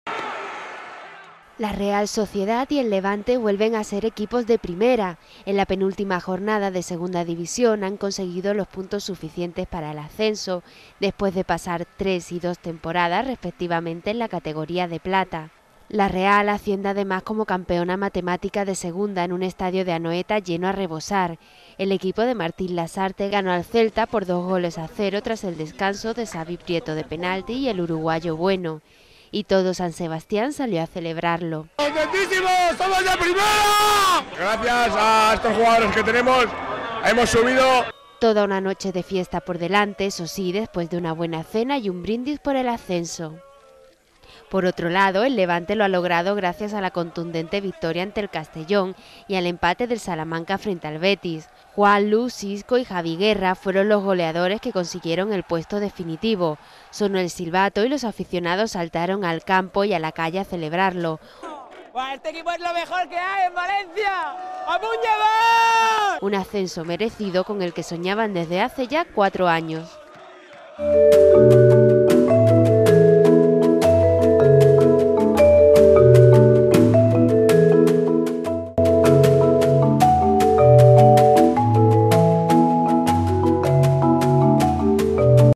El Levante y la Real, ya son equipos de Primera
- Los aficionados celebran en la calle el ascenso de sus equipos

Redacción, 14 junio (CERES TELEVISIÓN)

La Real Sociedad y el Levante vuelven a ser equipos de Primera. En la penúltima jornada de Segunda División han conseguido los puntos suficientes para el ascenso después pasar tres y dos temporadas, respectivamente, en la categoría de plata.

La Real asciende además como campeona matemática de Segunda en un estadio de Anoeta lleno a rebosar. El equipo de Martín Lasarte ganó al Celta por 2-0 con goles tras el descanso de Xabi Prieto de penalti y el uruguayo Bueno. Y todo San Sebastián salió a celebrarlo. Toda una noche de fiestas por delante, eso sí, después de una buena cena y un brindis por el ascenso. Por otro lado, el Levante lo ha logrado gracias a la contundente victoria ante el Castellón y al empate del Salamanca frente al Betis. Juanlu, Xisco y Javi Guerra fueron los goleadores que consiguieron el puesto definitivo. Sonó el silbato y los aficionados saltaron al campo y a la calle a celebrarlo. Un ascenso merecido con el que soñaban desde hace cuatro años.